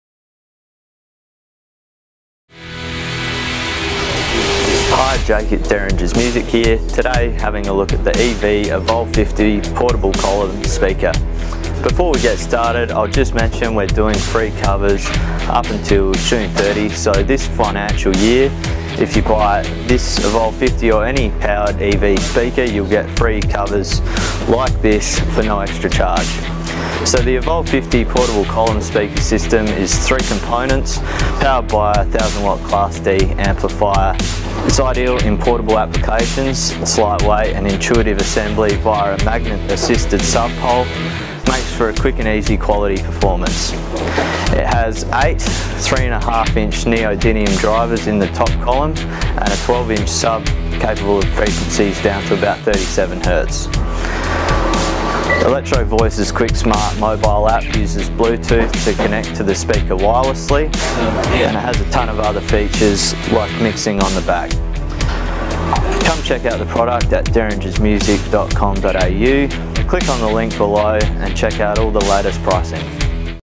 EV Evolve 50 Portable Column System
Electro-Voice Evolve 50 Portable Column System: quickly and wirelessly configure, control, and monitor up to six EVOLVE 50 systems simultaneously, and receive immediate notification during critical performance conditions.

Ultra-wide 120° horizontal coverage via custom designed waveguides and array-formed 40 asymmetrical vertical coverage means all acoustical energy is directed towards both sitting and standing audiences.